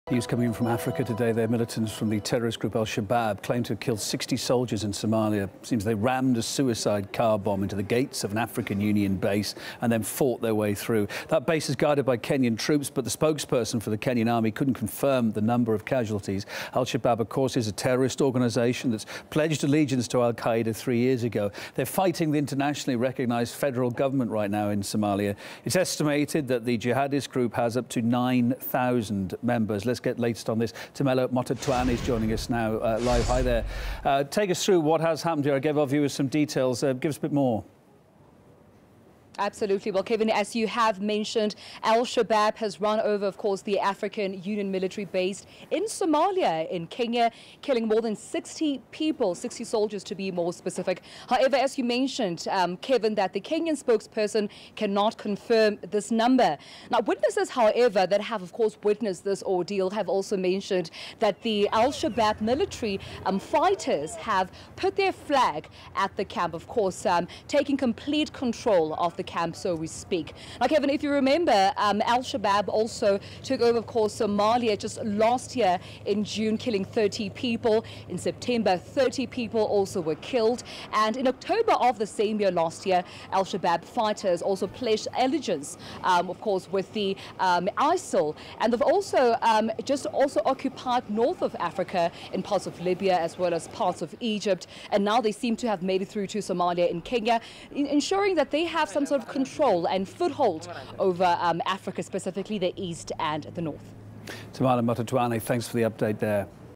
Al-Shabaab Attacks Military Base In Somalia, Kills 60 Soldiers
January 15, 2016 - In Africa, militants from the terrorist group al-Shabaab claim to have killed 60 soldiers in Somalia.